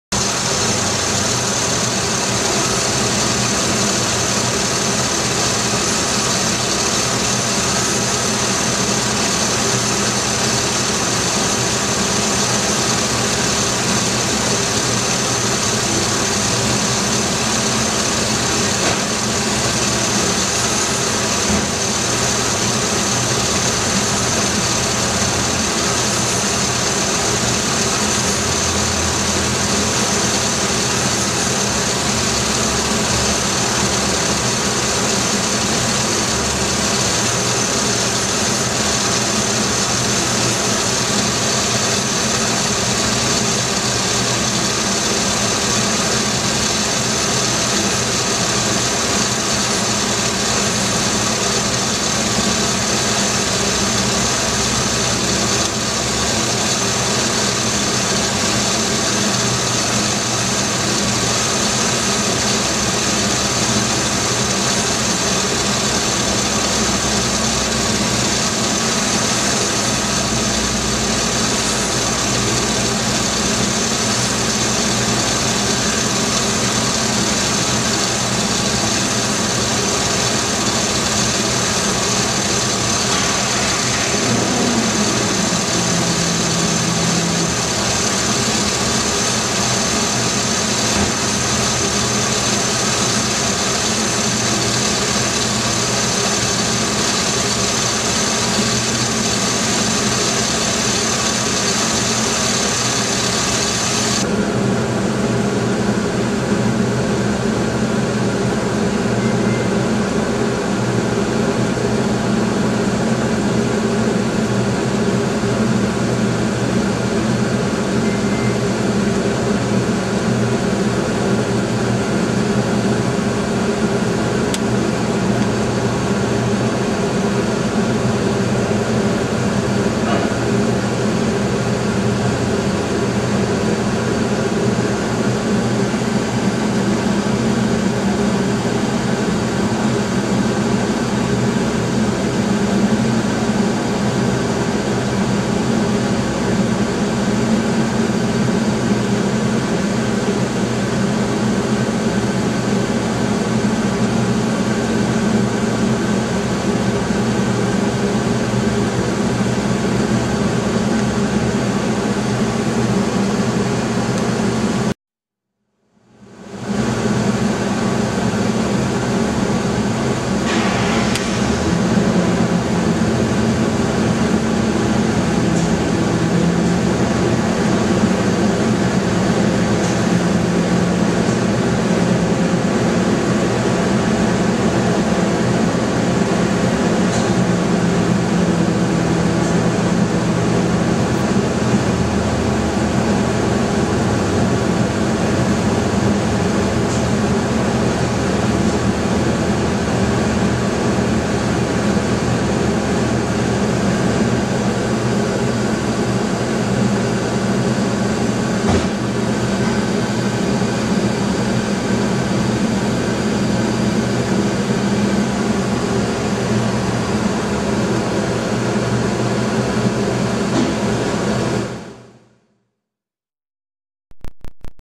GM 3.1 coolant leak.wmv
low coolant level, engine over heating, pressure test cooling system